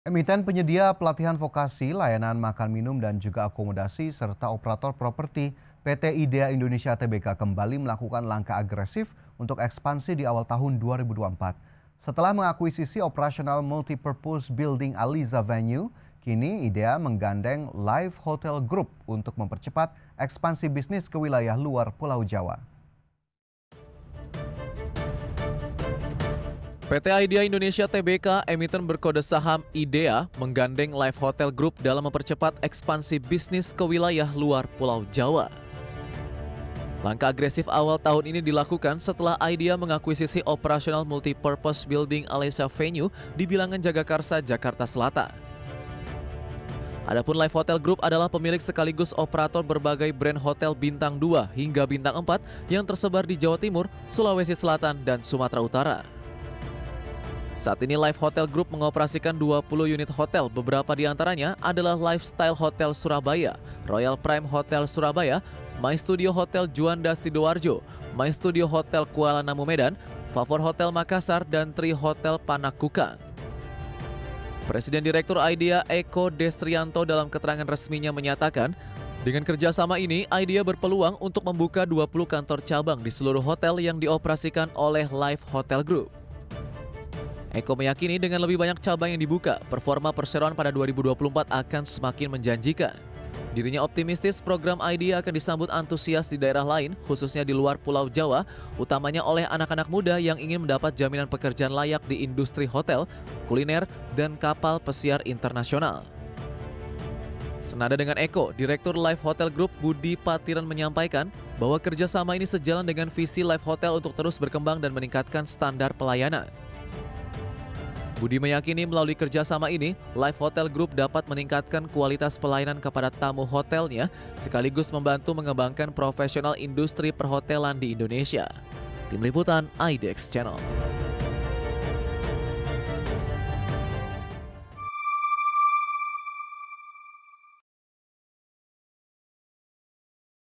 Expansi ke Makassar, IDEA Indonesia Teken Kerjasama Dengan Life Hotel Group - IDEA INDONESIA
16 Januari 2024 - Di awal 2024, IDEA INDONESIA menggandeng Life Hotel Group untuk membuka Cabang ke wilayah Makassar. Dengan kerjasama tersebut, IDEA akan membuka kantor cabang untuk mengoperasikan bisnis pelatihan vokasional non formal bidang hospitality dengan waktu pelatihan yang sangat singkat.

IDEA optimis, Program-program pelatihan yang disediakan akan disambut antusias di daerah lain khususnya di luar Pulau Jawa, utamanya oleh anak-anak muda yang ingin mendapat jaminan pekerjaan layak di industri hotel, restoran dan kapal pesiar internasional.

Life Hotel Group merupakan pemilik sekaligus operator beberapa hotel bintang 2 hingga bintang 4 yang tersebar di Jawa Timur, Sulawesi Selatan dan Sumatra Utara. Secara keseluruhan Life Hotel Group telah mengoperasikan 20 hotel.

Direktur Life Hotel Group Budi Patiran yakin melalui kerjasama dengan IDEA, Life Hotel Group dapat meningkatkan kualitas pelayanan kepada tamu hotelnya, sekaligus membantu mengembangkan profesional industri perhotelan di Indonesia.

“Kami ingin maju bersama dengan IDEA melalui kerja sama yang mengedepankan sharing resources, transfer of knowledge, dan inovasi program vokasi yang membawa dampak positif bagi masyarakat dan bisnis kami,” ujar Budi.

For more detail :
Atus Solihah as Manager Training Program Idea Indonesia